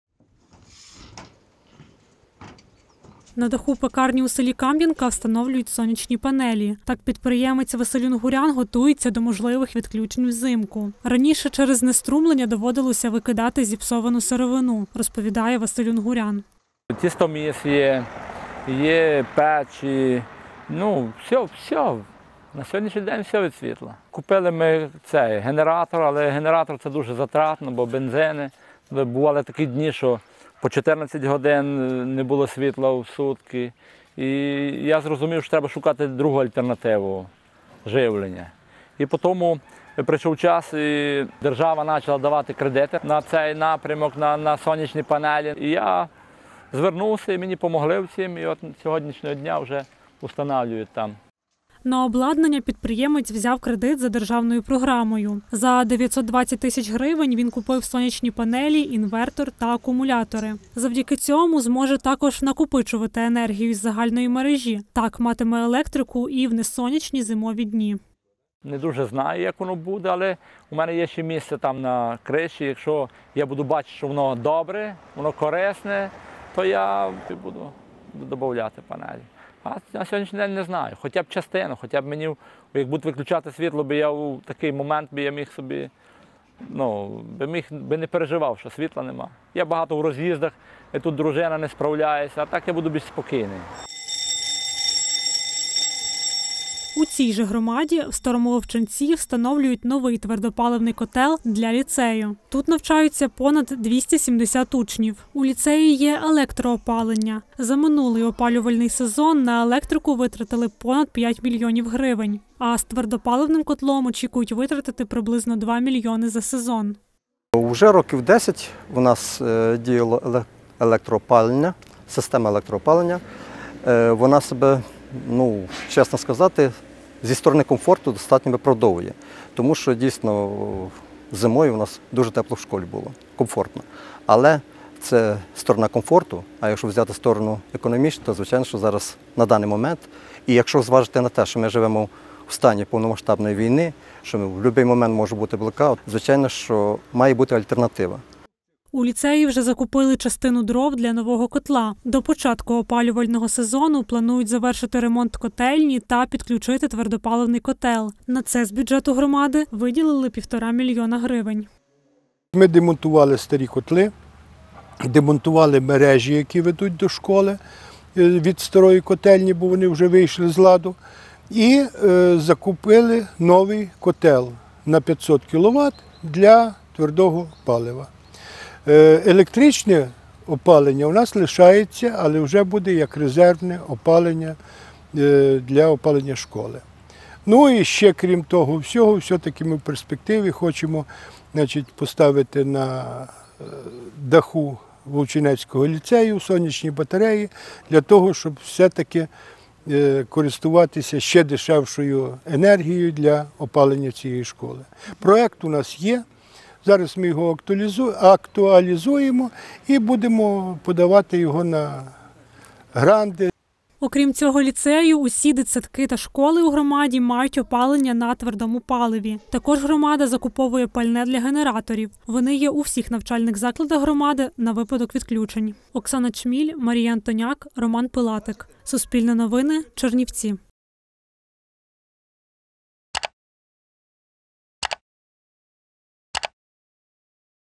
Котел на твердому паливі та кредит на сонячні панелі. Як громада на Буковині готується до блекаутів
Камʼянецька громада на Буковині — одна з тих, де і частково бізнес, і майже всі комунальні установи готуються до можливих відключень світла. Наприклад, власник пекарні встановлює сонячні панелі, а всі навчальні заклади мають генератори та твердопаливні котли. Монтаж останнього планують завершити до холодів. Скільки витрачають на альтернативні джерела живлення та опалення – у відео.